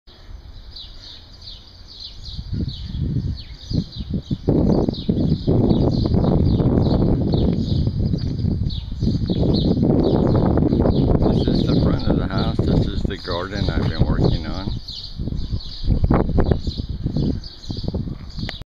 Mission House Update, Norman Oetker Protestant Christian Missionary, April, 2011, Reynosa Mk."
Mission House update, April, 2011 Reynosa Mexico, Norman Oetker Protestant Christian Missionary, L.A.M. "The Light Amidst the Mong" Christian Outreach, 1 Peter 2:9, Mae Hongson Thailand, Reynosa Mexico, English Class, St. Charles Missouri US. now serving Reynosa Mexico.
YouTube Bible Class,
This Bible Class 1, "Invasion of the Garden of Eden and Reynosa Mexico," is the beginning of many to come I pray, for an on-going series on Christian Basics.